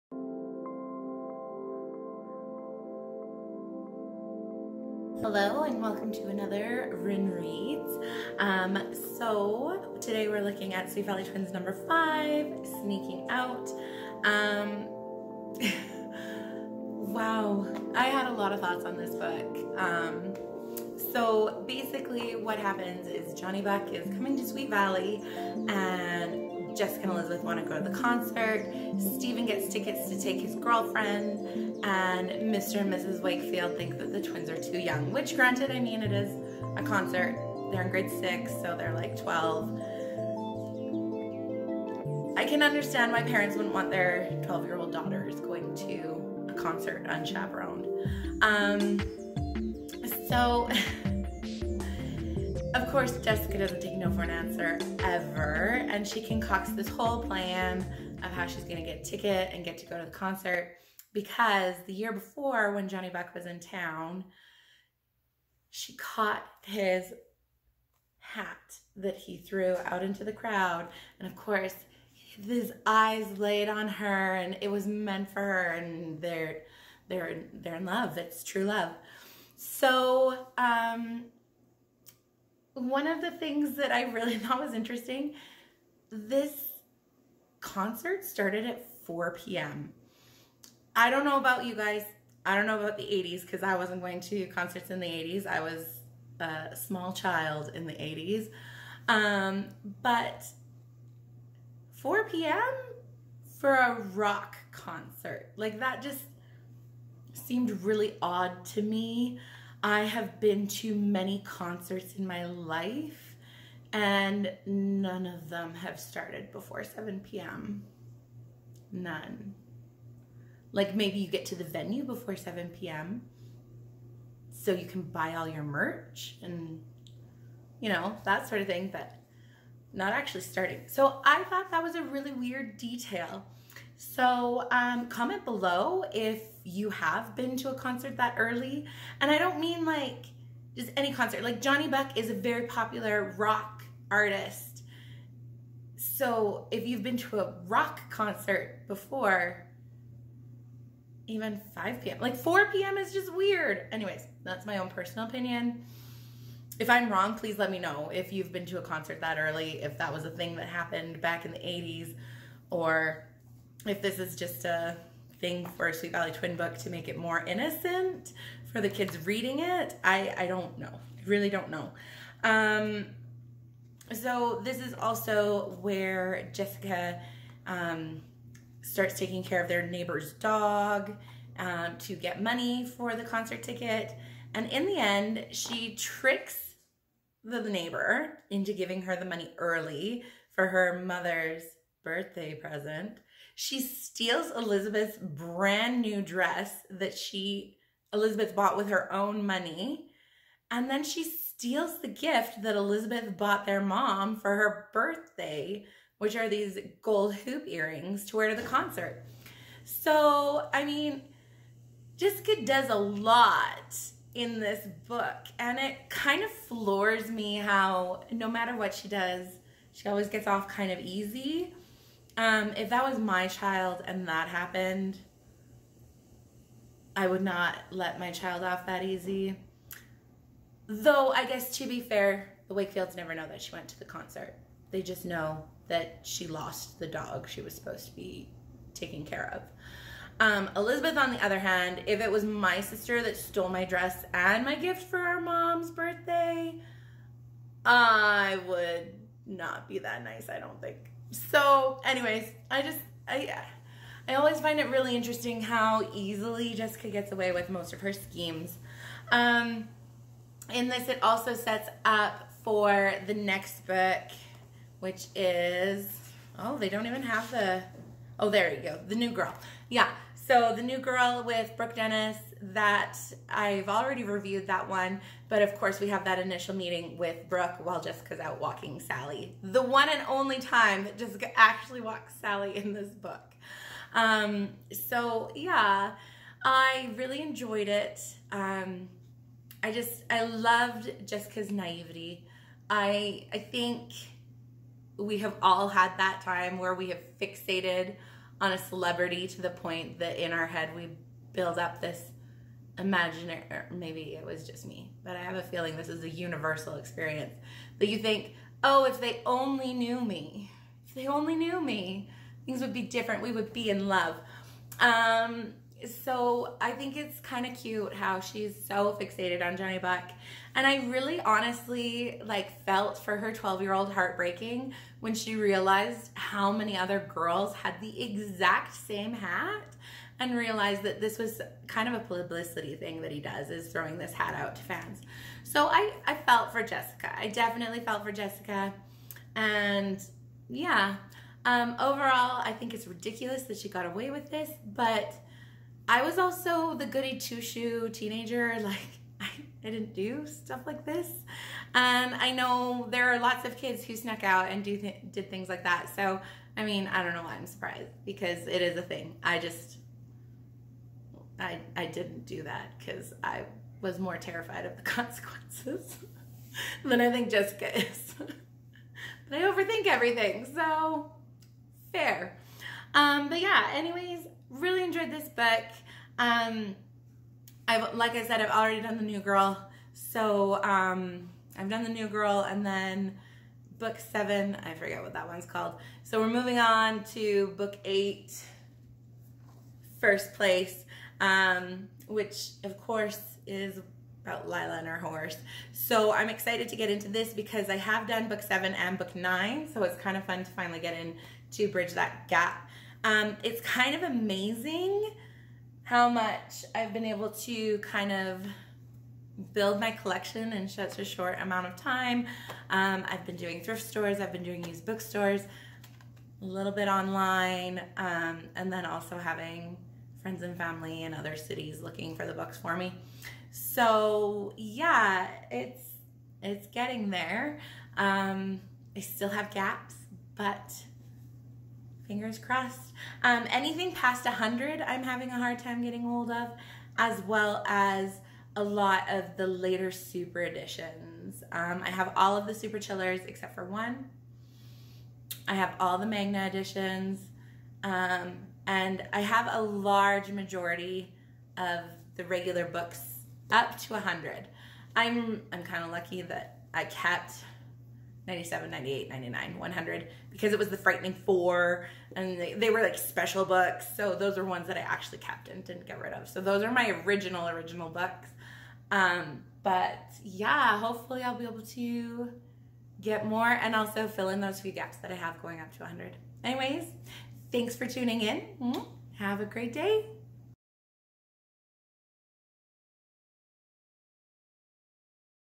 Sweet Valley Twins 5 - Francine Pascal
I have lots of feeling about Jessica going to a Johnny Buck concert.